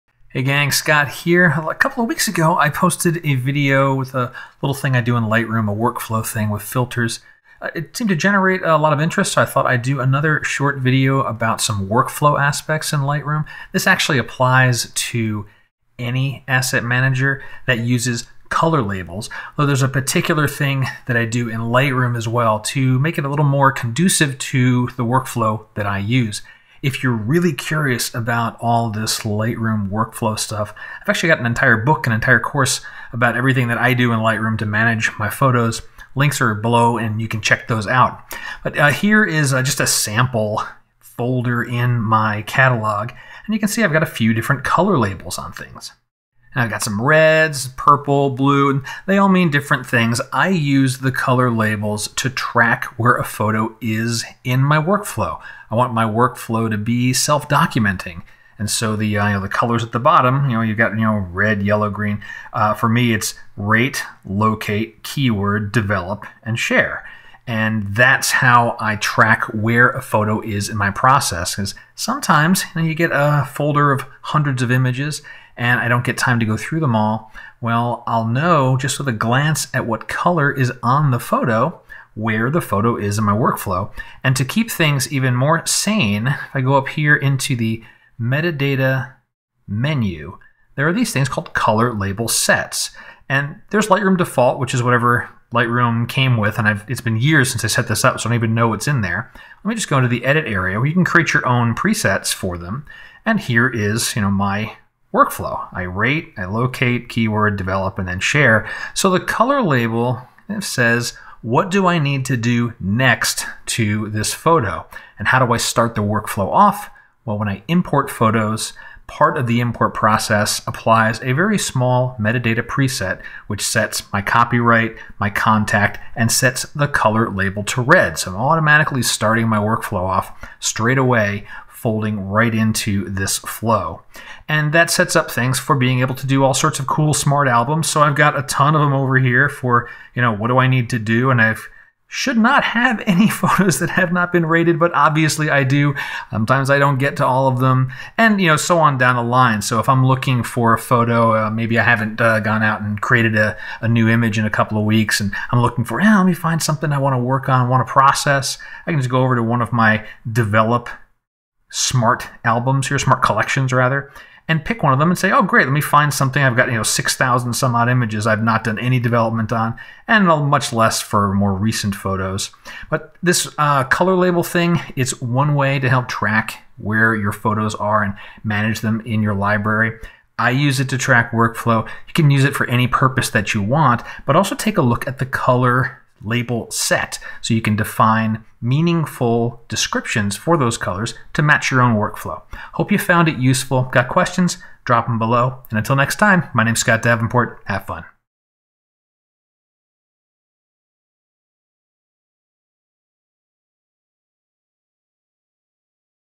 Color Labels And Color Label Sets In Lightroom
I use Color Labels in Lightroom to track where a photo is in my overall workflow process. This technique works for other asset managers as well. In Lightroom, there’s an extra bonus! Add descriptive names using a Color Label Set.